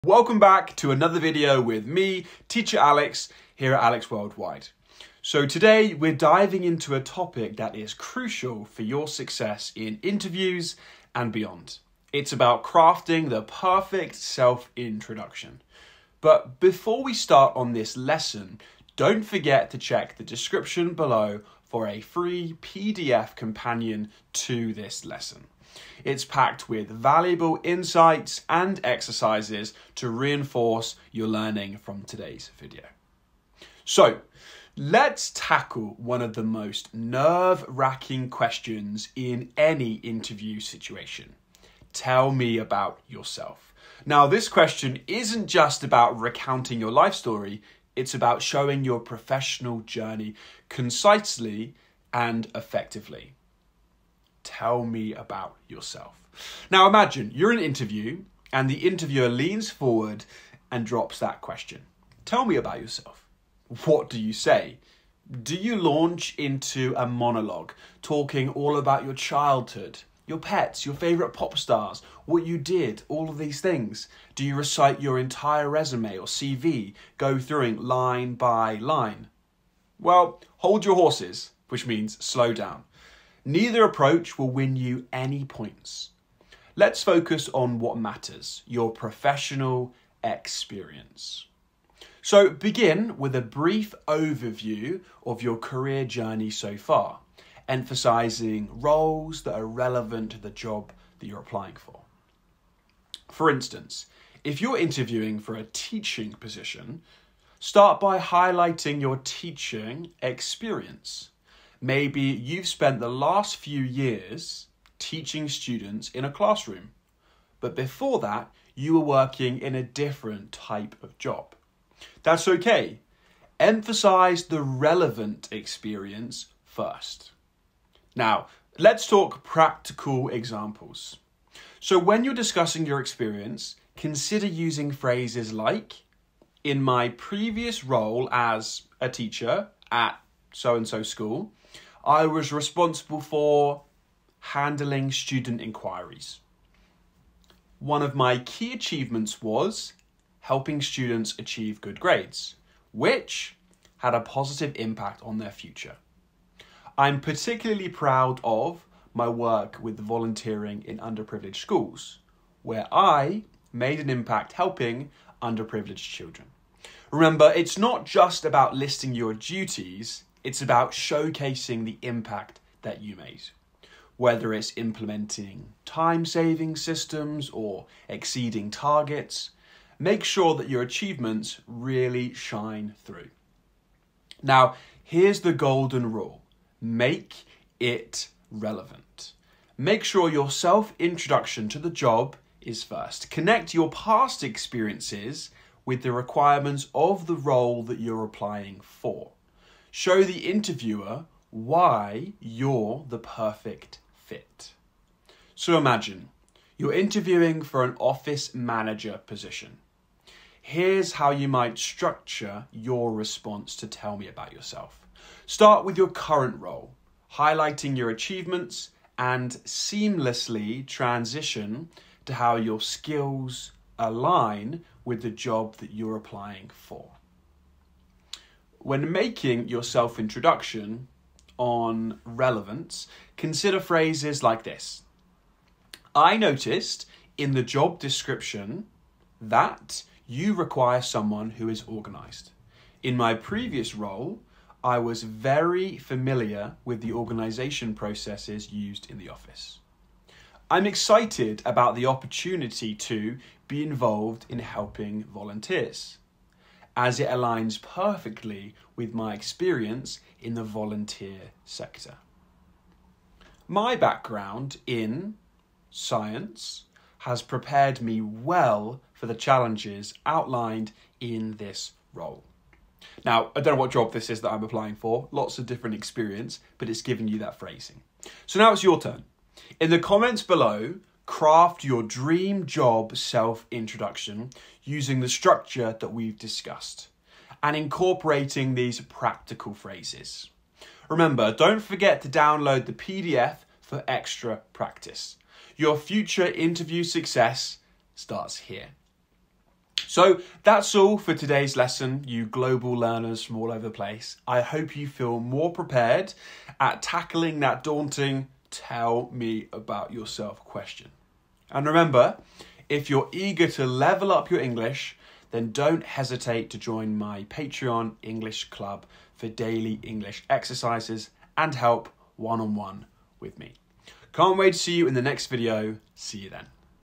Mastering the Job Interview: How to Answer 'Tell Me About Yourself' in English!
Welcome to AlexWorldwide! In this video, we're diving into the art of mastering the job interview by crafting the perfect self-introduction. Whether you're a seasoned professional or just starting out, nailing your self-introduction can make all the difference in landing your dream job.

What You'll Learn:
* Understand why the "Tell Me About Yourself" question is crucial in job interviews.
* Learn how to structure your self-introduction to highlight your relevant work experience effectively.
* Explore practical examples and easy-to-use phrases to make your self-introduction memorable.

Companion PDF:
Don't forget to download the companion PDF for this lesson! It's packed with additional tips, practical examples, and exercises to help you refine your self-introduction skills.

Get Started Today:
Watch the video, download the PDF, and take the first step towards mastering the job interview!